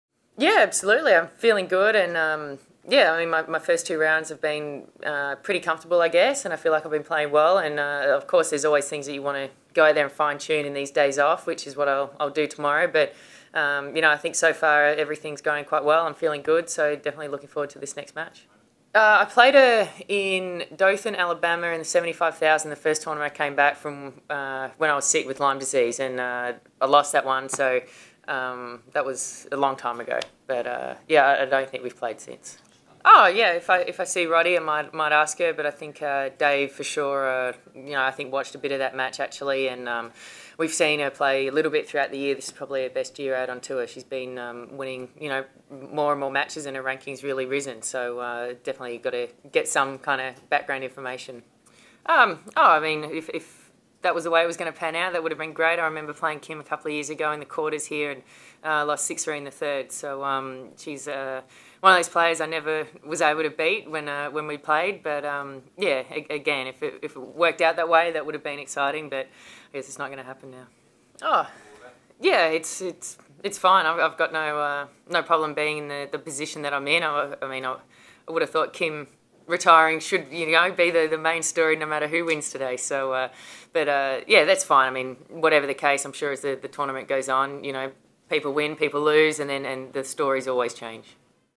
Sam Stosur - 2nd Round Press Conference US Open 2012
Sam Stosur has continued her sparkling run at US Open 2012, dropping just three games en route to victory over Edina Gallovits-Hall of Romania.